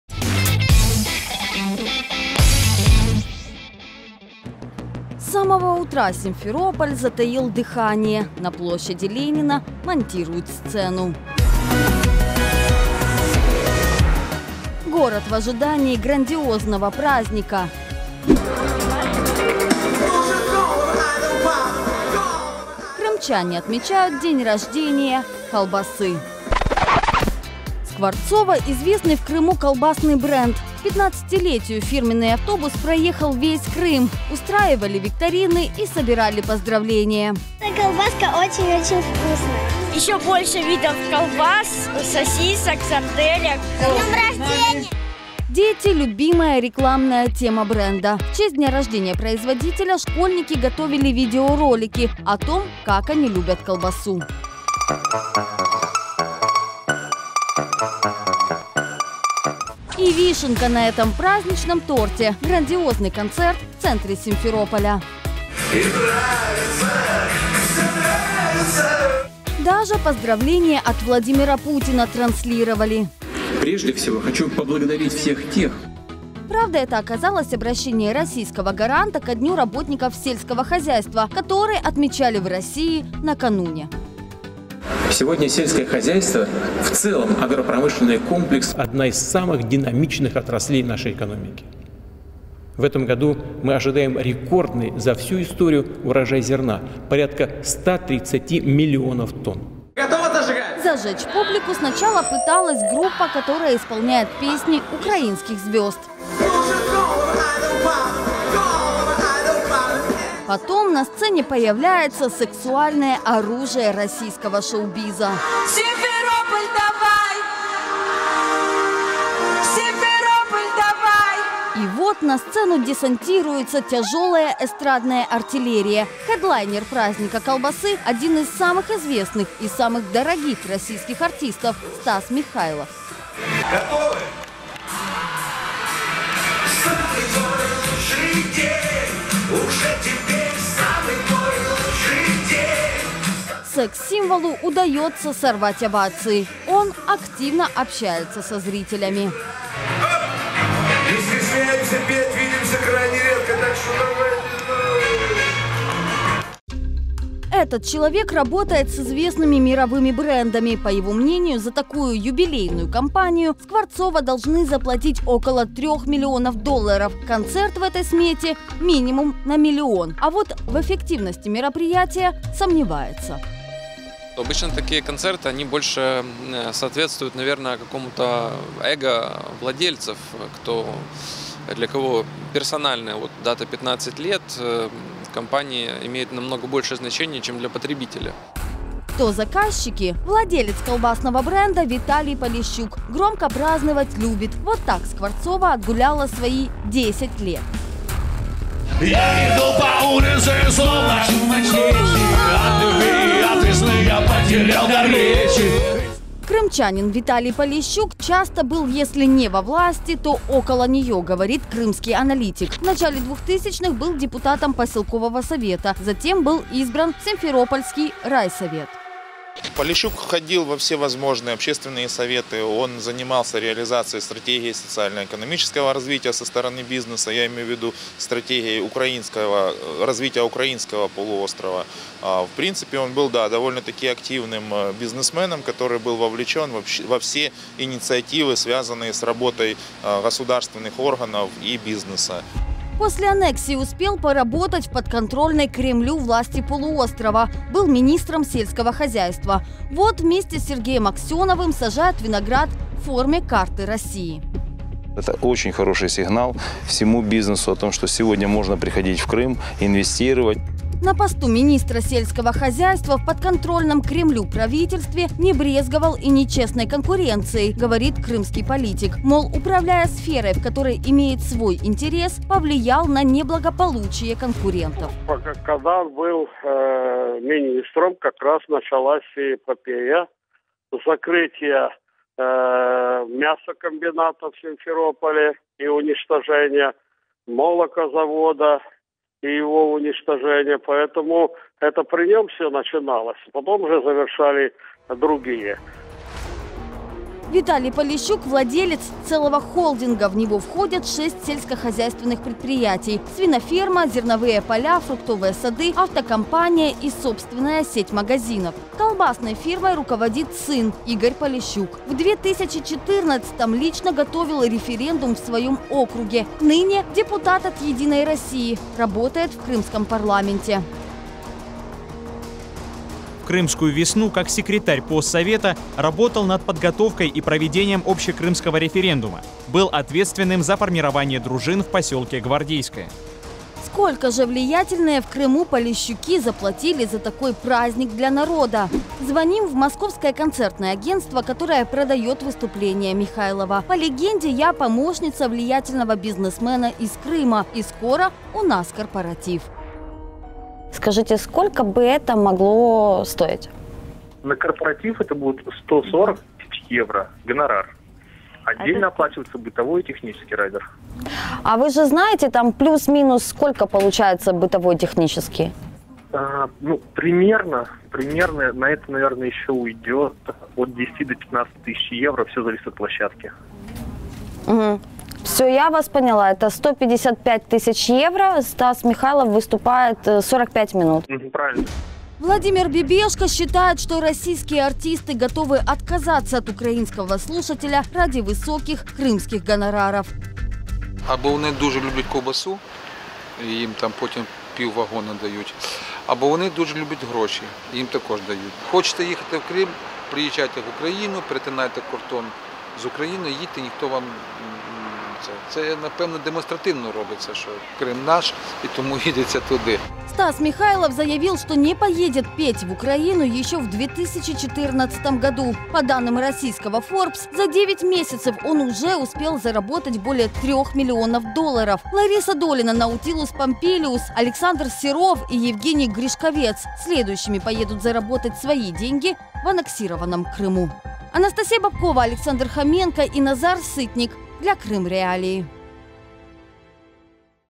Что общего между Стасом Михайловым и колбасой?
Тысячи зрителей, поздравления Владимира Путина и очень много колбасы. В Крыму отпраздновали юбилей колбасного бренда «Скворцово». Крым.Реалии выяснили, сколько стоит выступление Стаса Михайлова и как владельцы бренда связаны с подконтрольной Кремлю властью полуострова.